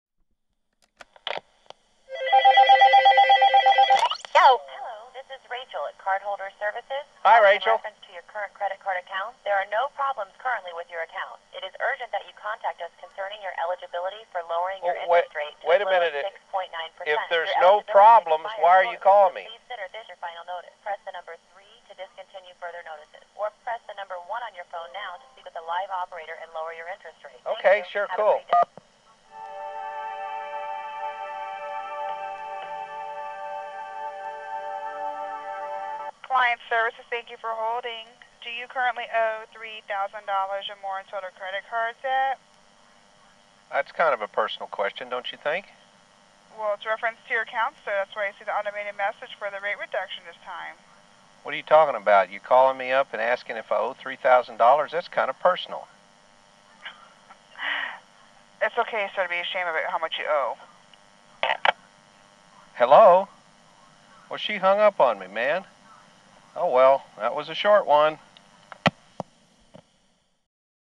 Telemarketer Personal Question
Telemarketers don't really like the victim asking questions.  This lady just didn't know what to say, so not only did she ask a person question, but, she got personal when it did not go according to script.